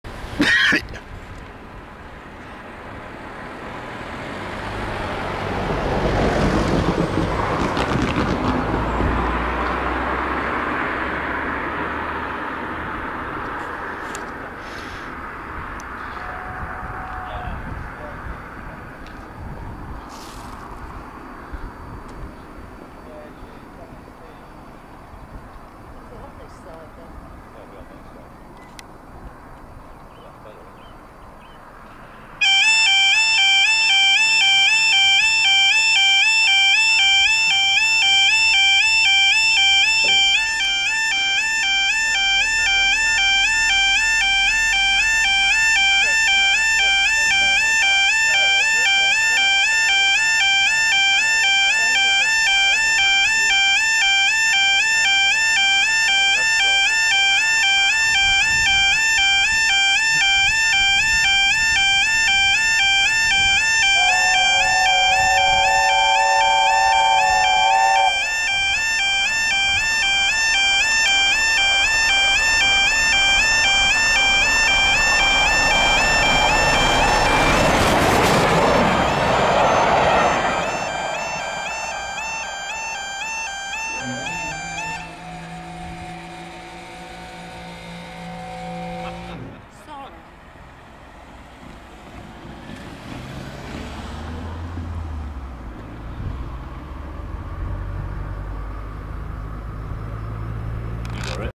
LNER A4 CLASS No 4464 BITTERN AT BROOME LANE CROSSING EAST GOSCOTE
Filmed at Broome Lane crossing East Goscote we see LNER A4 Class No 4464 Bittern transfering from Barrow Hill to Southall London via Harringworth Viaduct